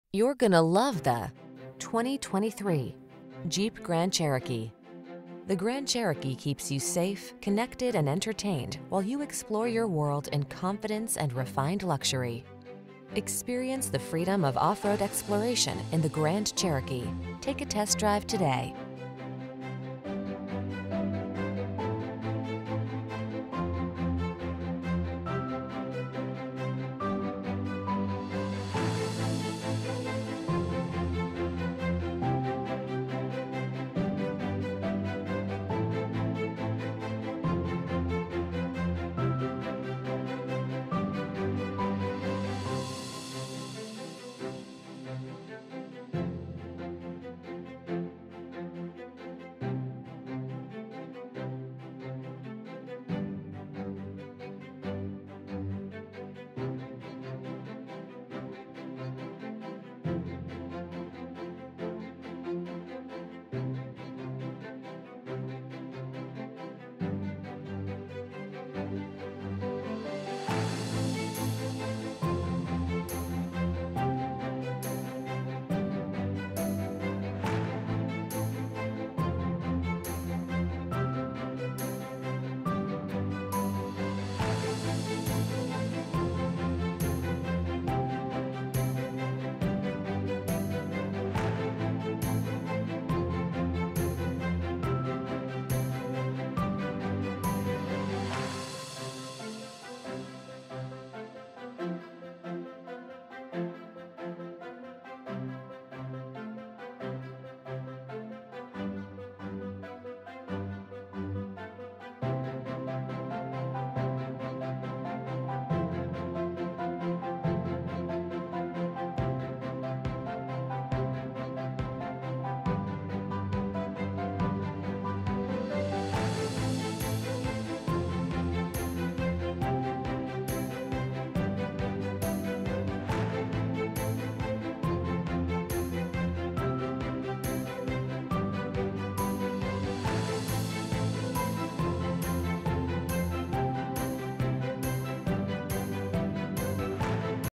2023 JEEP GRAND_CHEROKEE_L Ft. Worth, Dallas, Arlington, Irving P8821642
Baltic Gray Metallic Clearcoat New 2023 JEEP GRAND_CHEROKEE_L Limited available in 9501 South Fwy, Fort Worth, Texas at Meador CDJR. Servicing the Ft. Worth, Dallas, Arlington, Irving, TX area.

2023 JEEP GRAND_CHEROKEE_L Limited - Stock#: P8821642

Meador CDJR
9501 South Fwy